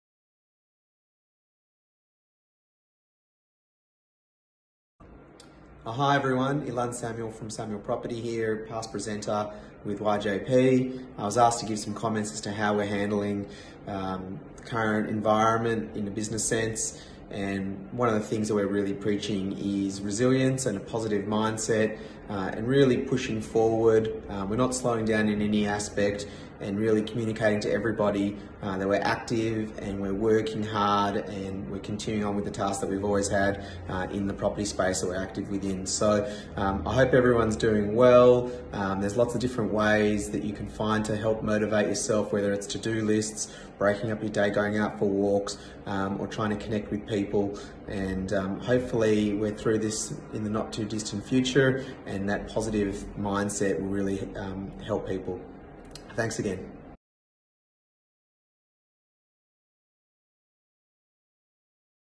Illan Samuel | Isolation Inspirations
Hosting our next Isolation Inspiration video installment, pleased to welcome back one of our panelists from last year's Property and Real Estate Masterclass, Managing Director and Founder of Samuel Property, Illan Samuel.

Illan hasn't been sitting out the coronavirus pandemic on the sidelines and kept things on track, adapting to running his business as 'un-usual' with a resilient mindset and future goal focus.

Take a moment to hear Illan's simple to implement Mentor Motivation tips!